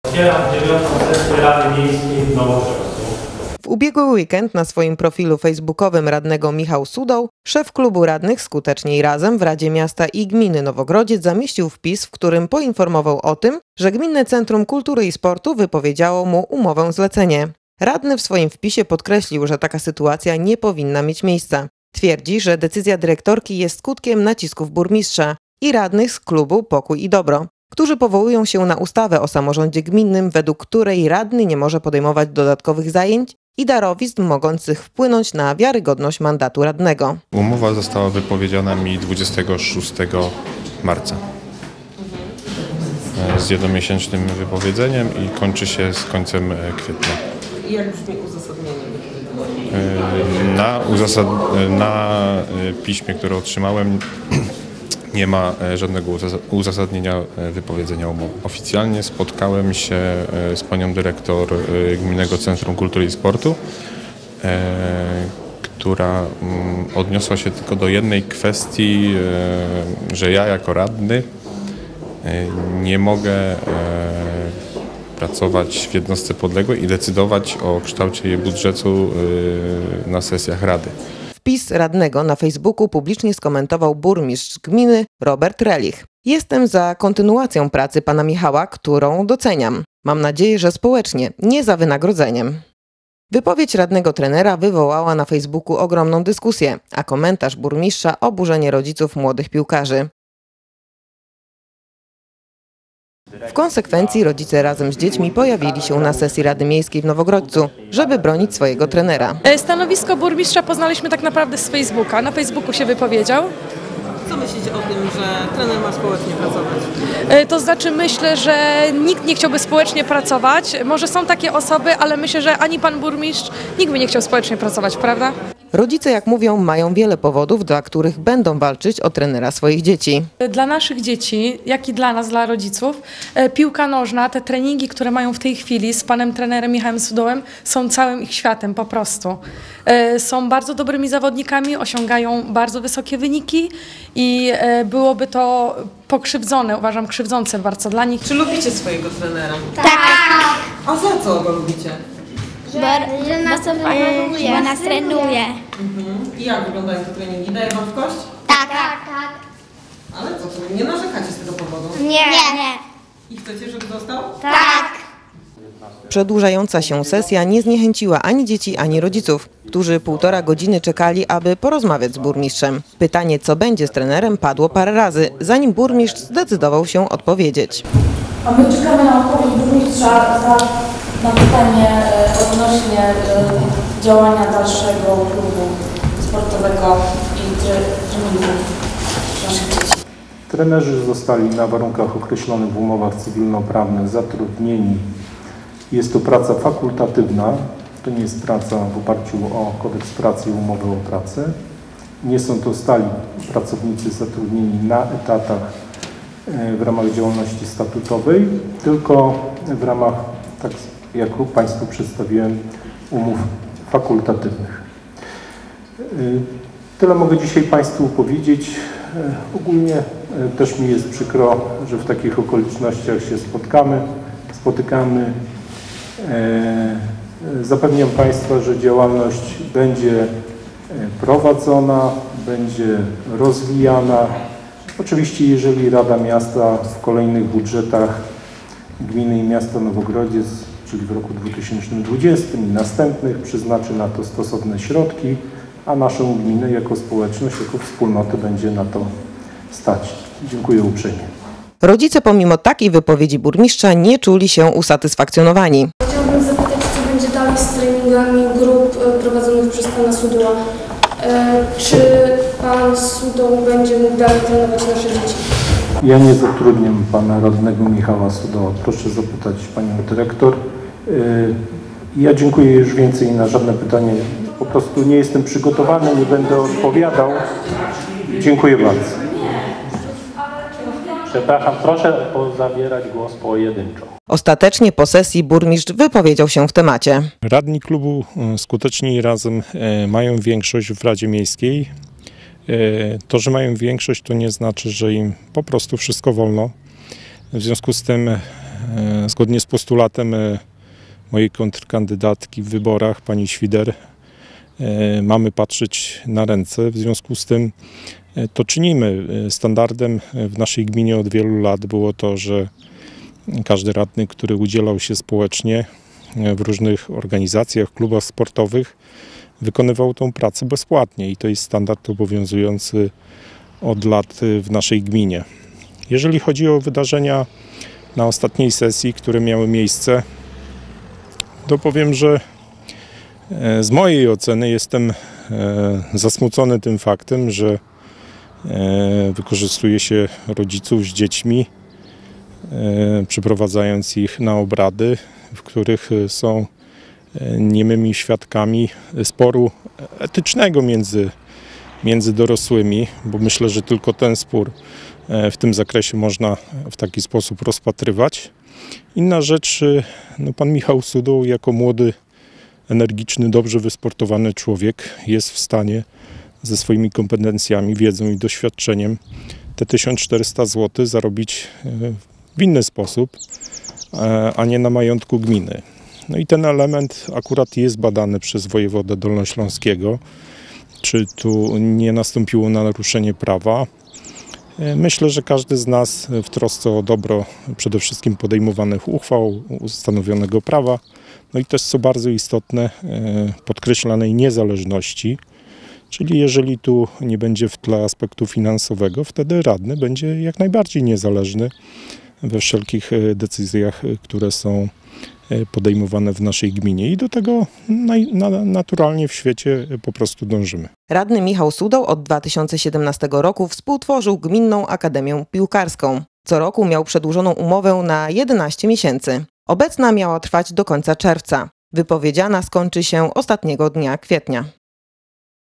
Walczą o radnego-trenera
W Nowogrodźcu radnemu nie przedłużono umowy na trenowanie dzieci. Burmistrz na jednym z internetowych profili, skwitował że powinien on pracować społecznie. Oburzeni rodzice  razem z dziećmi pojawili się na Sesji Rady Miejskiej w obronie swojego trenera.